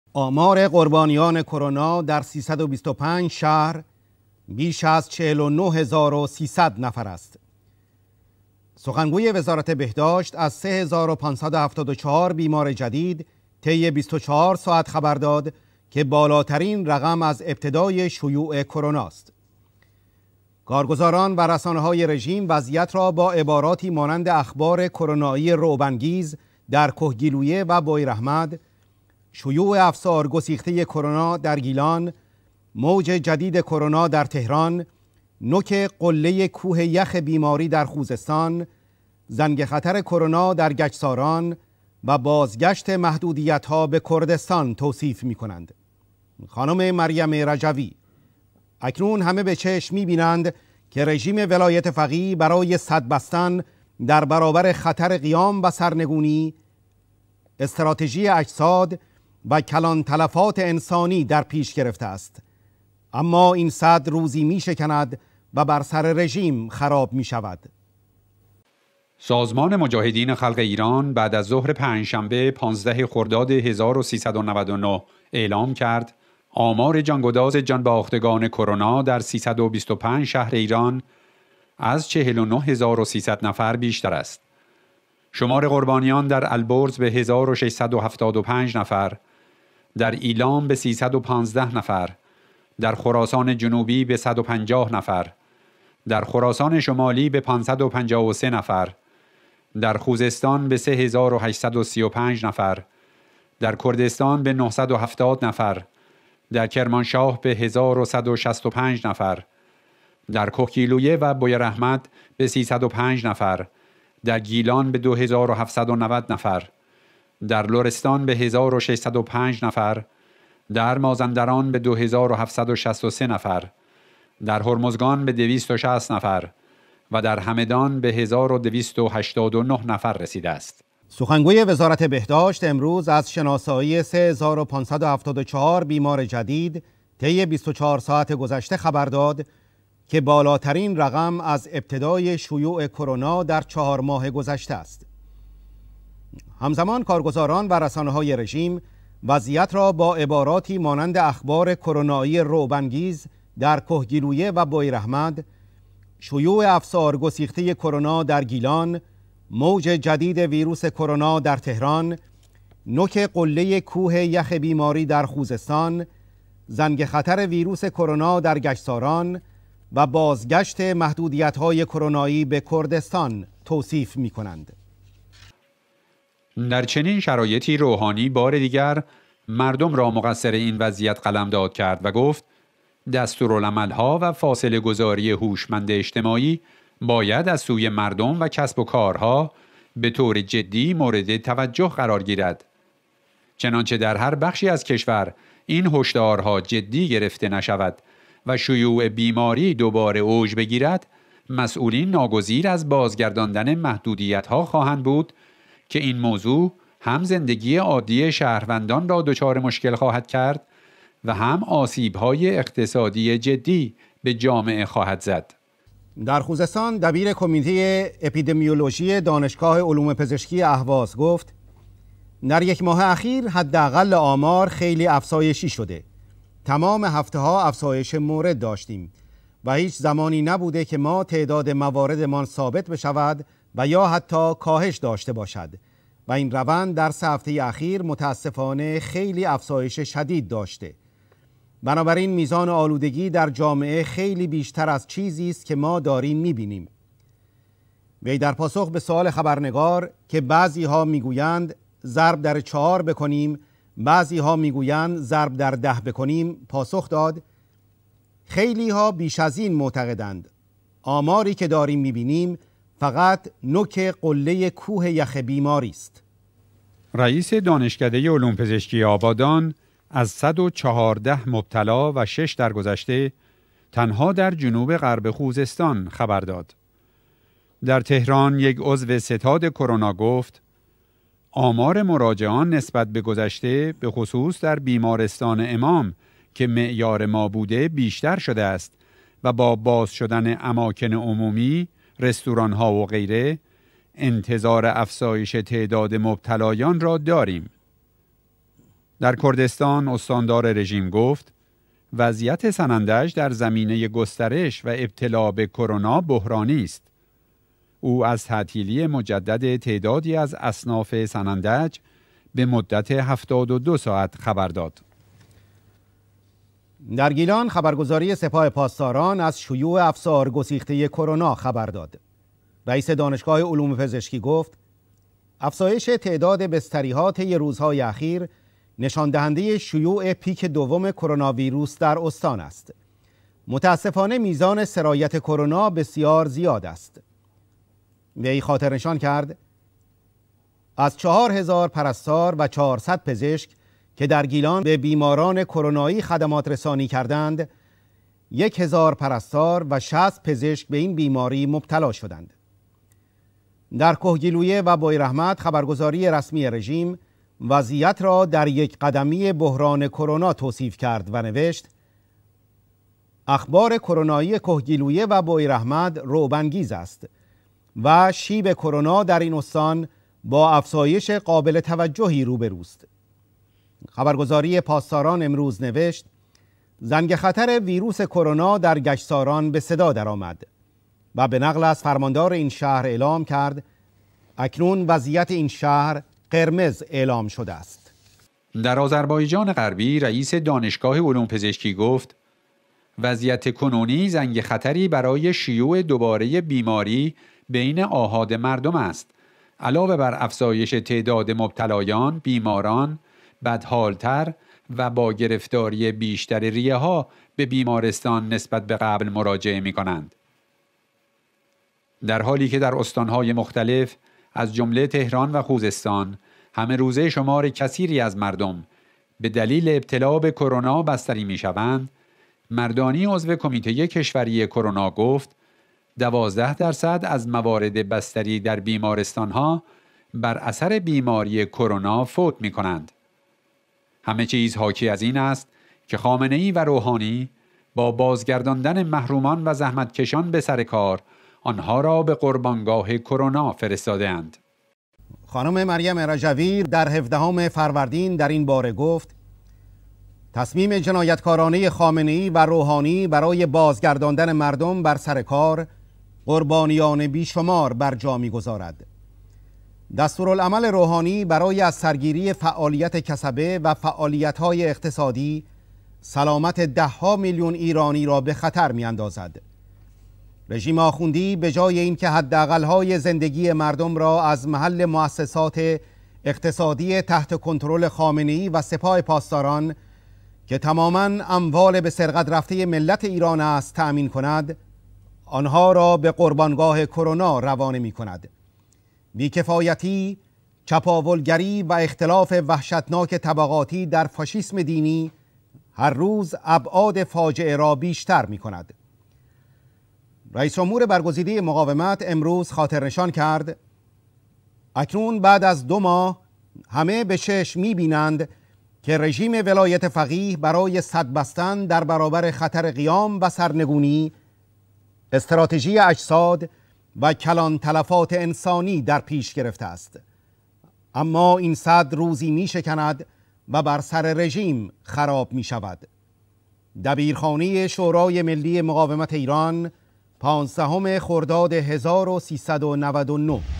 اطلاعیه شورای ملی مقاومت : آمار قربانیان کرونا در۳۲۵شهر بیش از۴۹هزار۳۰۰نفراست
سخنگوی وزارت بهداشت از ۳۵۷۴ بیمار جدید طی ۲۴ ساعت خبر داد كه بالاترين رقم از ابتداي شيوع كرونا است

 كارگزاران و رسانه هاي رژيم وضعيت را با عباراتي مانند اخبار کرونایی «رعب انگیز» درکهگیلویه و بویراحمد، «شيوع افسارگسيخته كرونا در گيلان» ، «موج جديد كرونا در تهران»، «نوك قله كوه يخ بيماري در خوزستان» ، «زنگ خطر كرونا در گچساران» و «بازگشت محدوديتها به كردستان» توصيف مي‌كنند

خانم مريم رجوي : اكنون همه به چشم مي‌بينند كه رژيم ولايت فقيه براي سد بستن در برابر خطر قيام و سرنگوني، استراتژي اجساد و كلان تلفات انساني در پيش گرفته ‌است اما اين سد روزي مي‌شكند و بر سر رژيم خراب مي‌شود

سازمان مجاهدين خلق ايران بعد از ظهر امروز پنجشنبه ۱۵ خرداد ۱۳۹۹ اعلام كرد، آمار جانگداز جانباختگان كرونا در ۳۲۵ شهر ايران از ۴۹ هزار و ۳۰۰ نفر بيشتر است. شمار قربانيان در البرز به ۱۶۷۵ نفر، در ايلام به ۳۱۵ نفر، در خراسان جنوبي به ۱۵۰ نفر، در خراسان شمالي به ۵۵۳ نفر، در خوزستان به ۳۸۳۵ نفر، در كردستان به ۹۷۰ نفر، در كرمانشاه به ۱۱۶۵ نفر، در كهكيلويه و بويراحمد به ۳۰۵ نفر، در گيلان به ۲۷۹۰ نفر، در لرستان به ۱۶۰۵ نفر، در مازندران به ۲۷۶۳ نفر، در هرمزگان به ۲۶۰ نفر و در همدان به ۱۲۸۹ نفر رسيده است.

سخنگوی وزارت بهداشت امروز از شناسایی ۳۵۷۴ بیمار جدید طی ۲۴ ساعت گذشته خبر داد كه بالاترين رقم از ابتداي شيوع كرونا در ۴ ماه گذشته است. همزمان كارگزاران و رسانه‌هاي رژيم وضعيت را با عباراتي مانند اخبار كرونايي «رعب‌انگيز درکهگیلویه و بویراحمد»، «شيوع افسارگسيخته كرونا در گيلان»، «موج جديد ويروس كرونا در تهران»، «نوك قله كوه يخ بيماري در خوزستان»، «زنگ خطر ويروس كرونا در گچساران» و «بازگشت محدوديتهاي كرونايي به كردستان» توصيف مي‌كنند.

در چنين شرايطي روحاني بار ديگر مردم را مقصر اين وضعيت قلمداد كرد و گفت: «دستورالعمل‌ها و فاصله‌گذاری هوشمند اجتماعی، باید از سوی مردم و کسب و کارها به طور جدی مورد توجه قرار گیرد.

 چنانچه در هر بخشی از کشور این هشدارها جدی گرفته نشود و شیوع بیماری دوباره اوج بگیرد، مسئولین ناگزیر از بازگرداندن محدودیت‌ها خواهند بود که این موضوع هم زندگی عادی شهروندان را دچار مشکل خواهد کرد و هم آسیب‌های اقتصادی جدی به جامعه خواهد زد»(ايرنا ۱۵ خرداد).

در خوزستان دبیر کمیته اپیدمیولوژی دانشگاه علوم پزشکی اهواز گفت: «در یک ماه اخیر حداقل آمار، خیلی افزایشی شده...تمام هفته‌ها افزایش مورد داشتیم و هیچ زمانی نبوده که ما تعداد مواردمان ثابت بشود و یا حتی کاهش داشته باشد و این روند در سه هفته اخیر متأسفانه خیلی افزایش شدید داشته بنابراین میزان آلودگی در جامعه خیلی بیشتر از چیزی است که ما داریم می‌بینیم». وي در پاسخ به سوال خبرنگار كه «بعضیها می‌گویند ضربدر ۴ بکنیم بعضی‌ها می‌گویند ضربدر ۱۰ بکنیم» پاسخ داد: «خیلی‌ها بیش از این معتقدند...آماري که داریم می‌بینیم فقط نوک قله کوه یخ بیماری است» (تلويزيون رژيم خوزستان ۱۳ خرداد). رئیس دانشکده علوم پزشکی آبادان از ۱۱۴ مبتلا و ۶ درگذشته تنها در جنوب غرب خوزستان خبر داد(خبرگزاري صدا و سيماي رژيم ۱۴ خرداد).

در تهران يك عضو ستاد کرونا گفت: «آمار مراجعان نسبت به گذشته به خصوص در بیمارستان امام که معیار ما بوده بیشتر شده است و با باز شدن اماکن عمومی، رستوران‌ها و… انتظار افزایش تعداد مبتلایان را داریم (رکنا ۱۵ خرداد).

در كردستان استاندار رژيم گفت: «وضعیت سنندج در زمینه گسترش و ابتلا به کرونا بحرانی است. او از تعطیلی مجدد تعدادی از اصناف سنندج... به مدت ۷۲ ساعت خبر داد» (جوان ارگان سپاه پاسداران ۱۵ خرداد).

در گيلان خبرگزاري سپاه پاسداران از «شيوع افسارگسيخته كرونا» خبر داد. رئیس دانشگاه علوم پزشکی گفت: «افزایش تعداد بستری ها طی روزهای اخیر نشان دهنده شیوع پیک دوم کرونا ویروس در استان است...متأسفانه میزان سرایت کرونا بسیار زیاد است». وي خاطرنشان كرد: از «۴ هزار پرستار و ۴۰۰ پزشک» كه در گيلان به بیماران كرونايي خدمات‌رسانی کردند «یک هزار پرستار و ۶۰ پزشک به این بیماری مبتلا شدند» (جوان ارگان سپاه پاسداران ۱۴خرداد).

در کهگیلویه و بویراحمد خبرگزاري رسمي رژيم  وضعيت را «در یک قدمی بحران کرونا» توصيف كرد و نوشت: «اخبار کرونایی کهگیلویه و بویراحمد رعب انگیز» است و «شیب کرونا در اين استان با افزایش قابل توجهی روبه رو است» (ايرنا ۱۵ خرداد). خبرگزاري پاسداران امروز نوشت «زنگ خطر ويروس كرونا در گچساران به صدا در آمد» و به نقل از فرماندار اين شهر اعلام كرد «اكنون وضعيت اين شهر قرمز اعلام شده است».

در آذربایجان غربی رئیس دانشگاه علوم پزشکی گفت وضعيت كنوني «زنگ خطری برای شیوع دوباره بیماری بین آحاد مردم است. علاوه بر افزایش تعداد مبتلایان، بیماران، بدحال‌تر و با گرفتاری بیشتر ریه‌ها به بیمارستان، نسبت به قبل مراجعه می‌کنند» (خبرگزاری صدا وسیماي رژيم ۱۵ خرداد).

در حاليكه در استانهاي مختلف از جمله تهران و خوزستان همه روزه شمار كثيري از مردم به دليل ابتلا به كرونا بستري ميشوند مردانی، عضو کمیته کشوری کرونا گفت:«۱۲ درصد از موارد بستری در بیمارستان‌ها بر اثر بیماری کرونا فوت می‌کنند» (جوان ارگان سپاه پاسداران ۱۴ خرداد).

همه چيز حاكي از اينست كه خامنه اي و روحاني با بازگرداندن محرومان و زحمتكشان به سركار آنها را به قربانگاه كرونا فرستاده اند.

خانم مريم رجوي در ۱۷ فروردين در اين باره گفت: «تصميم جنايتكارانه خامنه اي و روحاني براي بازگرداندن مردم بر سر كار قربانيان بيشمار بر جا ميگذارد. دستورالعمل روحاني براي از سرگيري ”فعاليت كسبه“ و ”فعاليتهاي اقتصادي“ سلامت دهها ميليون ايراني را به خطر مي اندازد. رژيم آخوندي... بجاي اينكه حداقل‌هاي زندگي مردم را از محل مؤسسات اقتصادي تحت كنترل خامنه اي و سپاه پاسداران